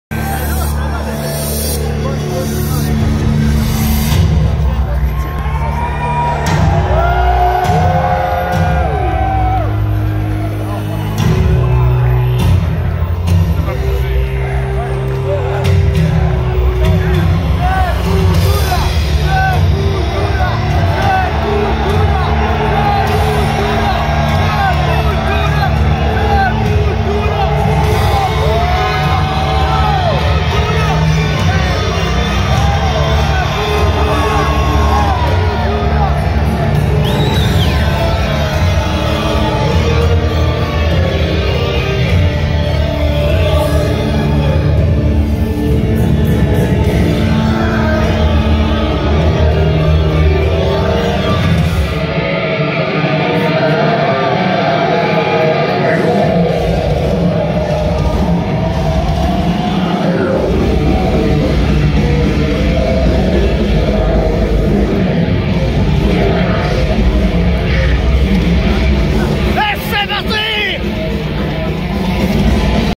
Kreator/Sepultura, Olympia, Montreal 9/06/23
The beginning of Sepultura's set at last night's Klash of the Titans Co-Headline show!